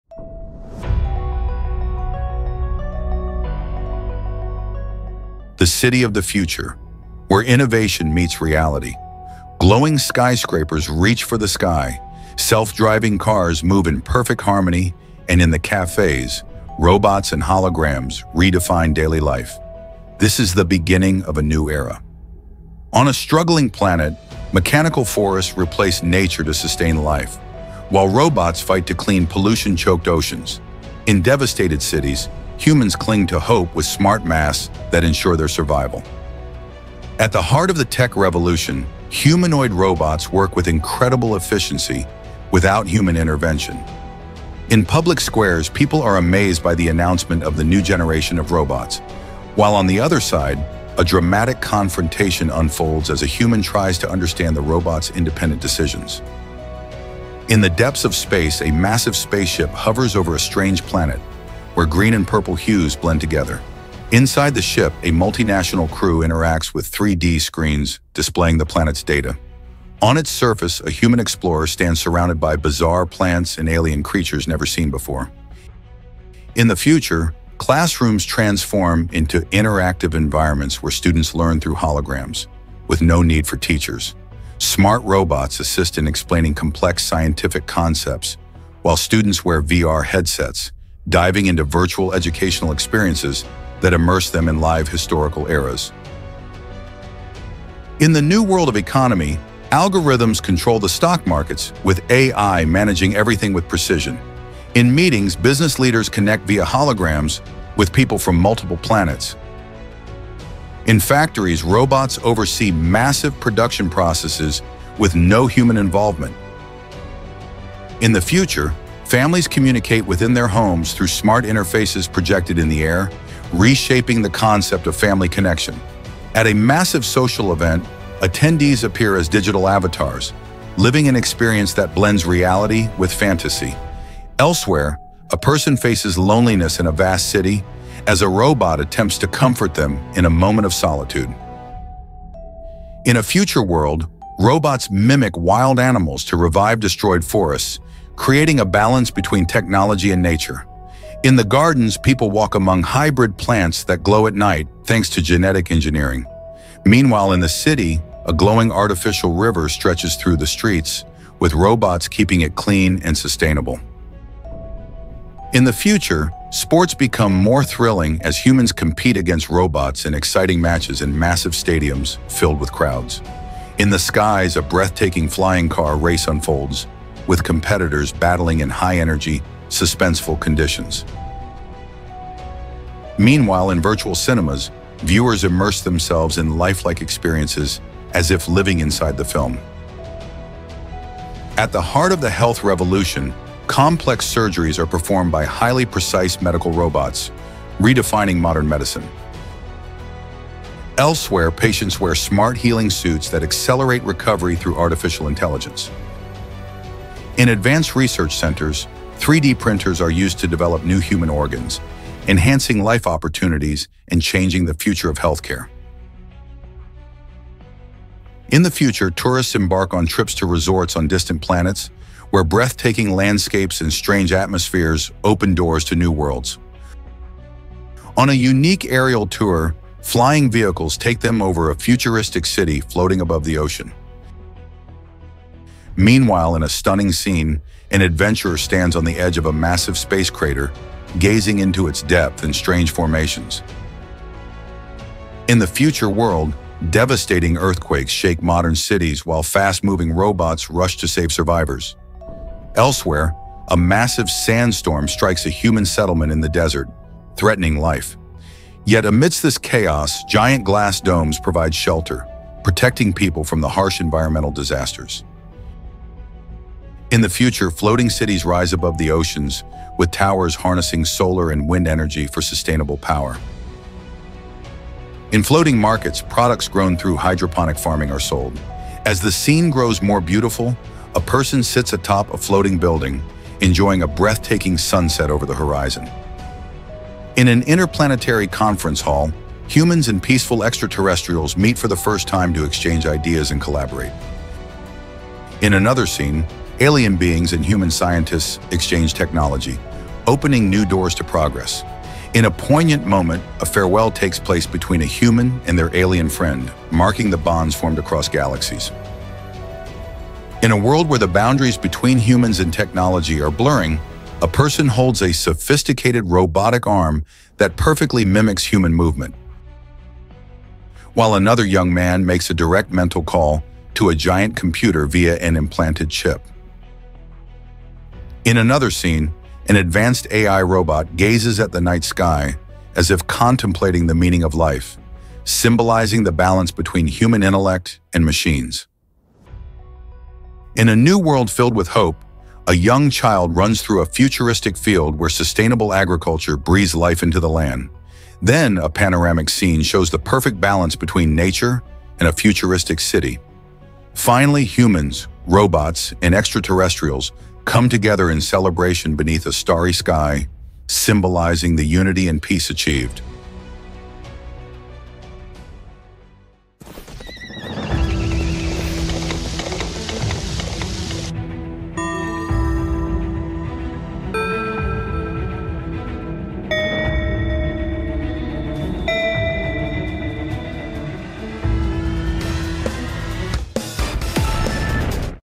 The Future of Humanity: A Vision Beyond 2050 | AI Short Film
This video embarks on an awe-inspiring journey through a futuristic vision that seamlessly blends technology, innovation, nature, and human interaction. From radiant cities driven by artificial intelligence and innovative solutions to global environmental crises, to the exploration of distant planets and their otherworldly ecosystems. It highlights advancements in education and healthcare, transformative shifts in work and economy, and profound connections between humans, robots, and extraterrestrial beings. Finally, it envisions a hopeful future, where humanity and technology achieve harmony with nature and urban landscapes. Each scene tells a unique chapter in the story of humanity’s inspiring and optimistic future.